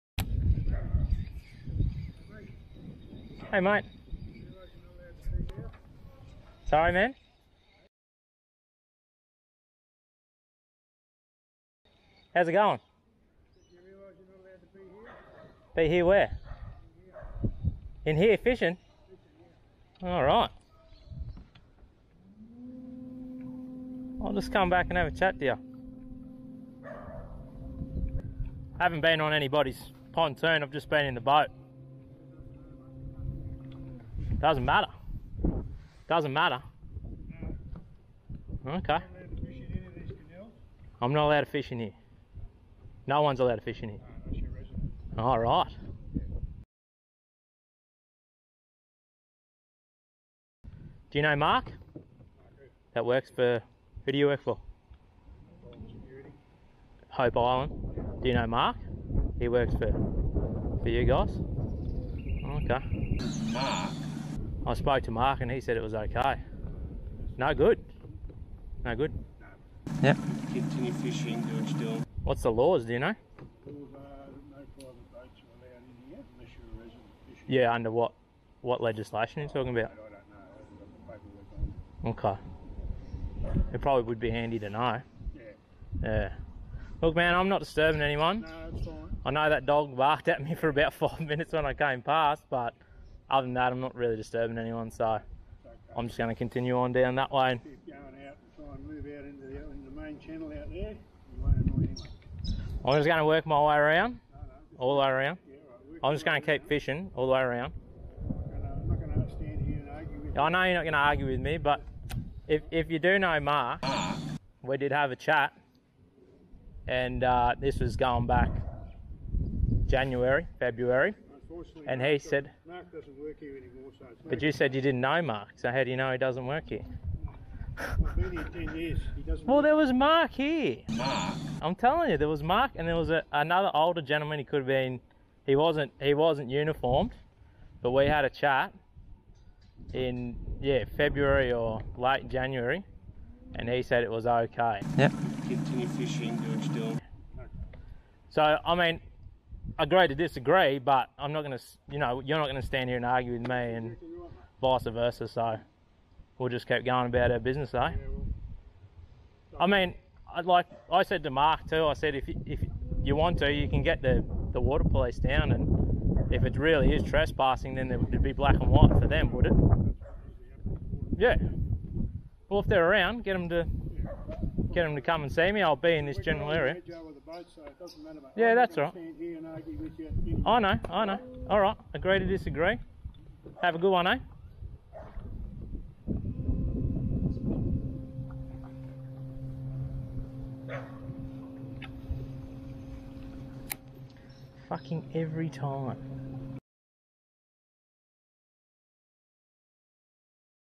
Fisherman VS Two Security guards!
This is in Australia, fishing in a coastal tidal waterway.